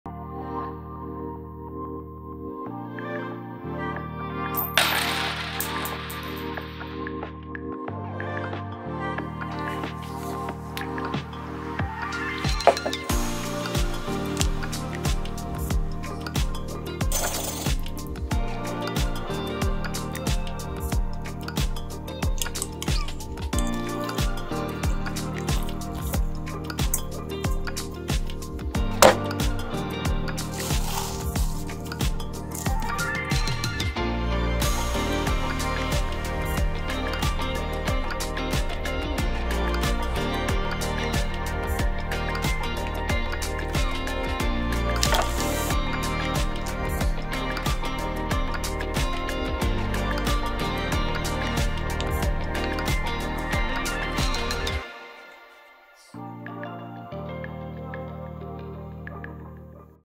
Pizza crudo e stracciatella
In occasione del World Pizza Day prepariamo una pizza gustosa che rende omaggio alla nostra italianità! Segui la ricetta per la pizza con prosciutto crudo e stracciatella di burrata!